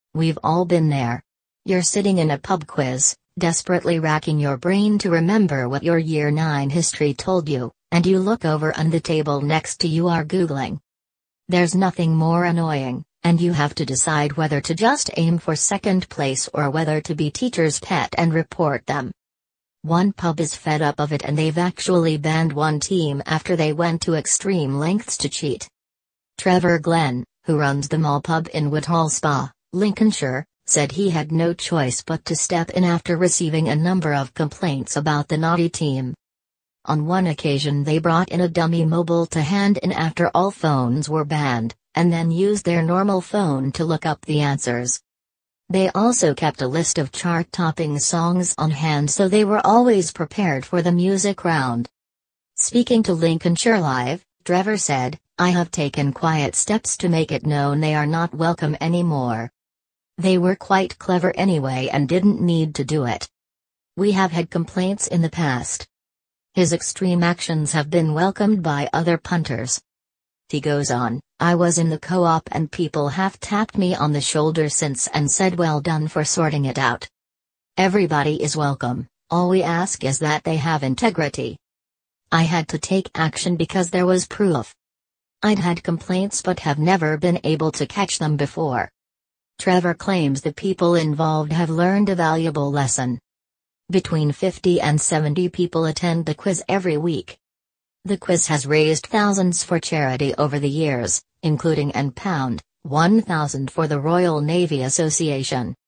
Landlord bans team from pub quiz after discovering their extreme cheating techniques
We've all been there. You're sitting in a pub quiz, desperately racking your brain to remember what your Year 9 history told you, and you look over and the table next to you are Googling.
There's nothing more annoying, and you have to decide whether to just aim for second place or whether to be teacher's pet and report them.